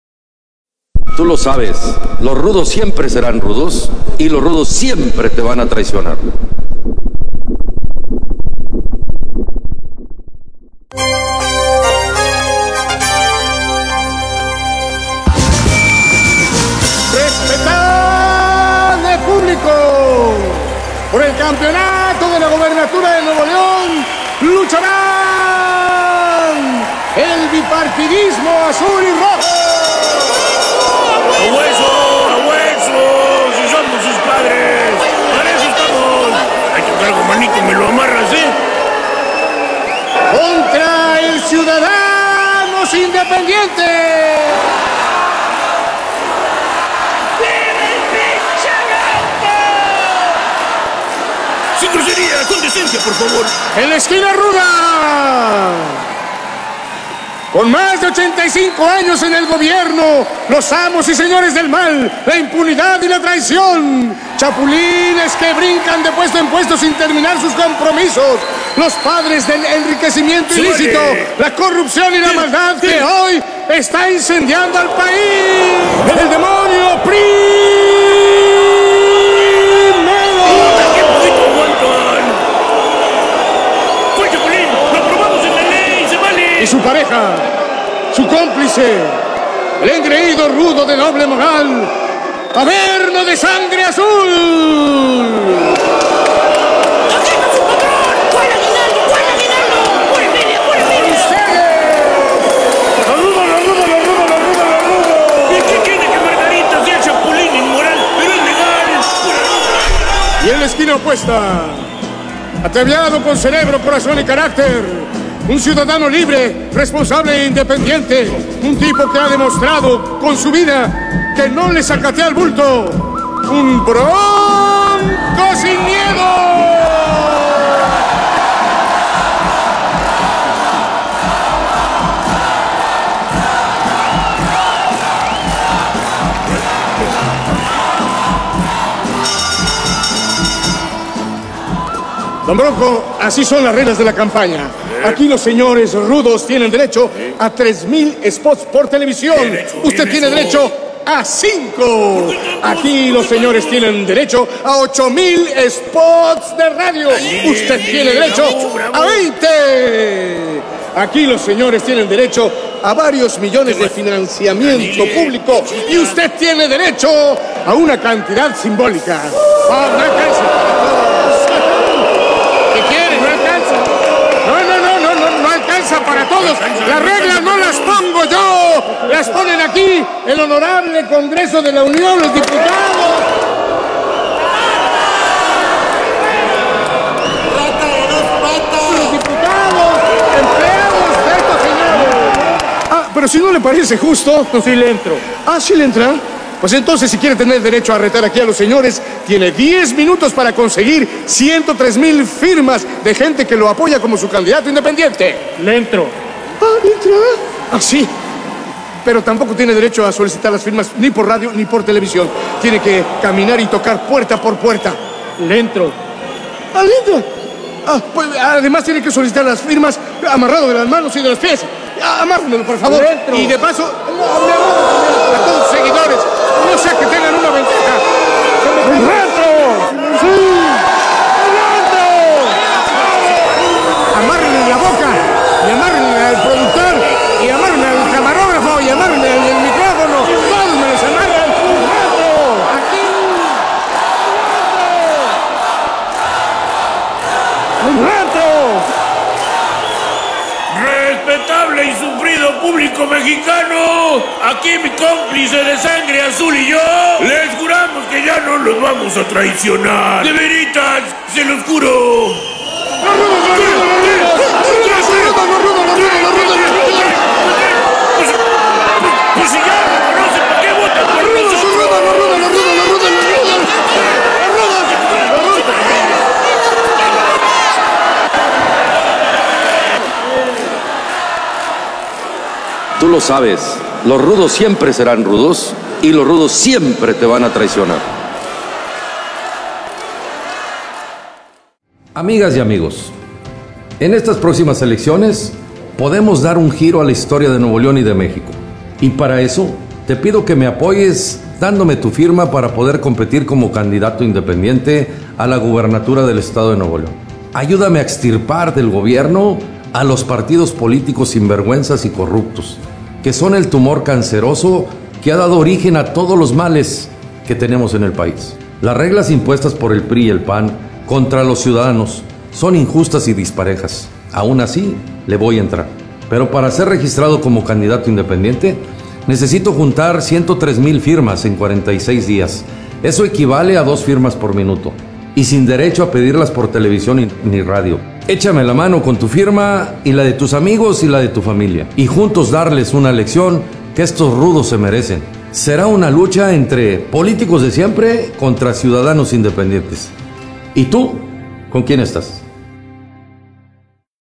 candidato gobernador Nuevo León 2015
Jaime Rodriguez Calderón
Bronco-Bronco
33 años en el PRI
ahora se lanza como candidato independiente.
Le interesa el bienestar animal